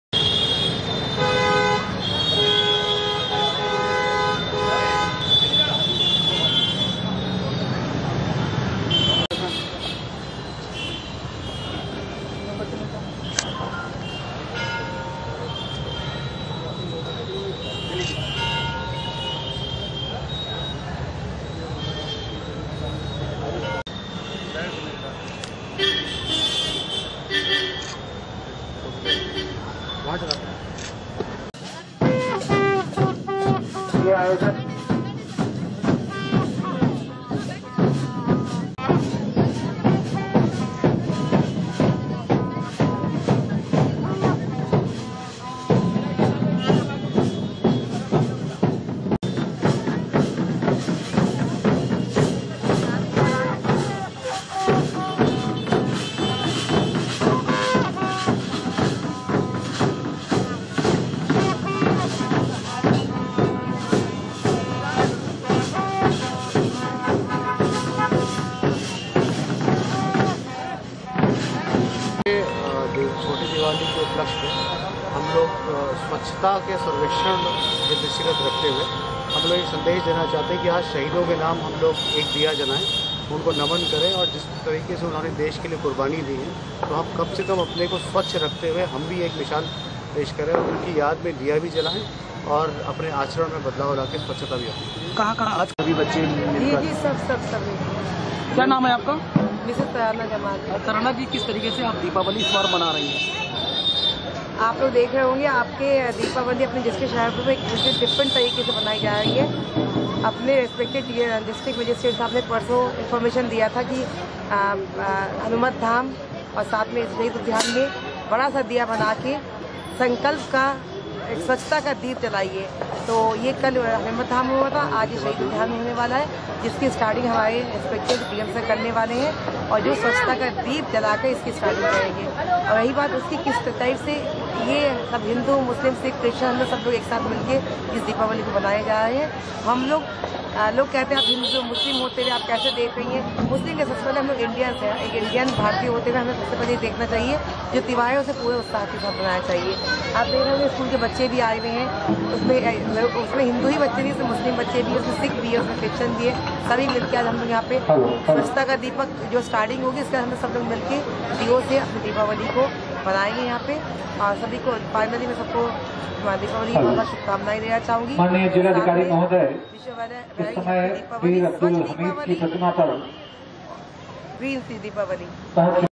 शाहजहांपुर: स्कूली बच्चों के साथ मिलकर डीएम ने जलाए 1001दीप, शहीदों को किया याद
शाहजहांपुर: डीएम अमृत त्रिपाठी ने दिवाली के अवसर पर स्वच्छ सर्वेक्षण 2019 के तहत जनपद को स्वच्छ और सुन्दर बनाने के लिए स्वच्छता संकल्प दीप अभियान चलाया. साथ ही डीएम एसपी ने शहीद उद्यान में शहीदों की प्रतिमाओं के समक्ष दीप जलाए. इस दौरान पार्क में स्कूली बच्चों ने 1001 दीप जलाने का कार्यक्रम किया.